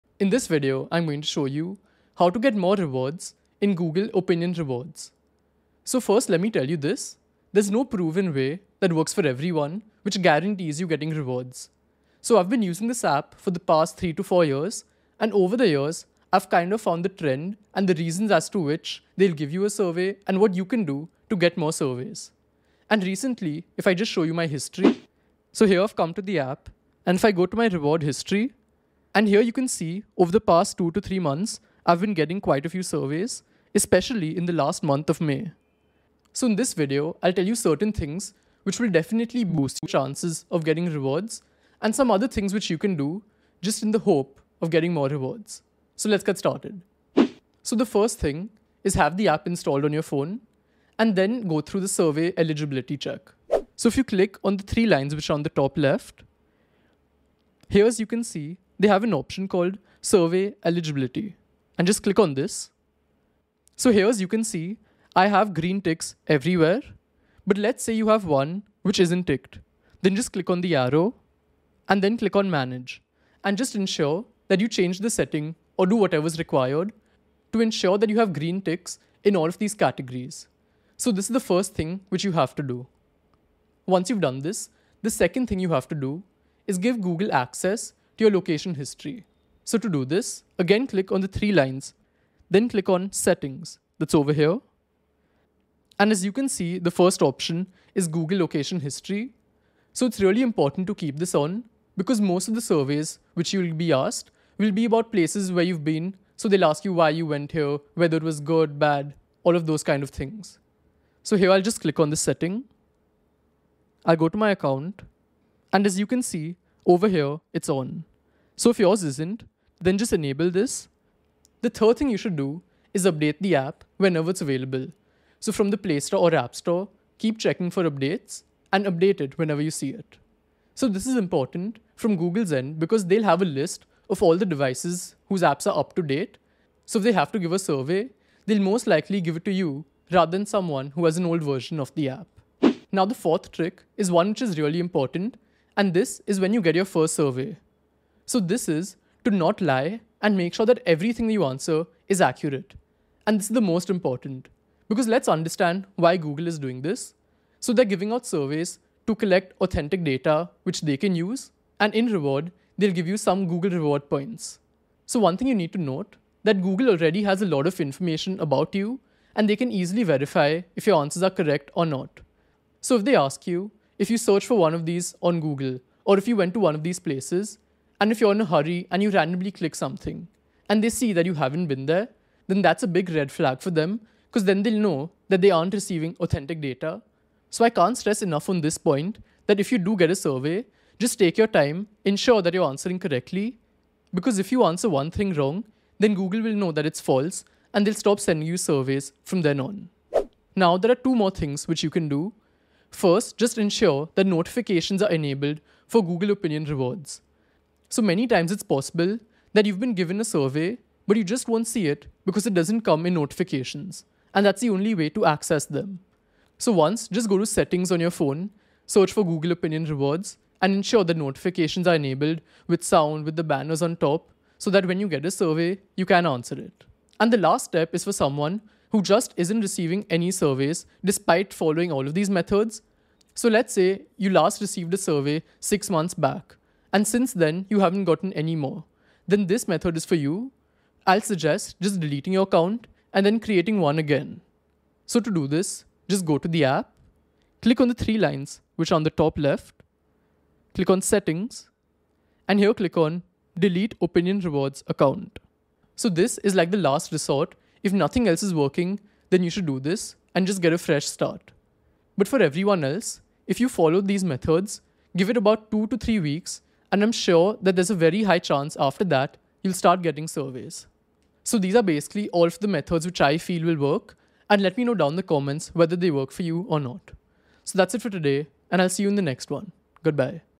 How To Get Surveys Faster in Google Opinion Rewards (Updated)
Boost Your Earnings: How to Get Surveys Faster on Google Opinion Rewards!

In this tutorial, we'll show you five effective methods to get surveys faster and increase your rewards.

Here's what you'll learn:

Fill Survey Eligibility: Discover how to complete your survey eligibility information accurately to receive more surveys.
Enable Location Access: Learn the importance of giving Google Opinion Rewards access to your location to increase the frequency of surveys.
Fill in Information Correctly: Ensure you're providing accurate information to avoid disqualification from surveys.
Avoid Lying on Surveys: Understand why it's crucial not to lie on surveys, as Google uses dummy surveys to filter out dishonest users. Trick questions might catch you, leading to fewer surveys and less money in the long run.
Update the App: Keep your app updated to the latest version to access all available surveys and features.
Enable Notifications: Make sure notifications are enabled so you never miss a survey opportunity.
Resetting a Dead Account: If your account isn't receiving surveys, consider deleting and re-creating your Google Opinion Rewards ID to reactivate survey opportunities.
By following these methods, you'll be able to get surveys faster and make the most out of Google Opinion Rewards, boosting your earnings effortlessly.
Timestamps:
00:00 - Introduction
00:46 - Trick 1
01:23 - Trick 2
02:02 - Trick 3
02:23 - Trick 4
03:20 - Trick 5 & Extra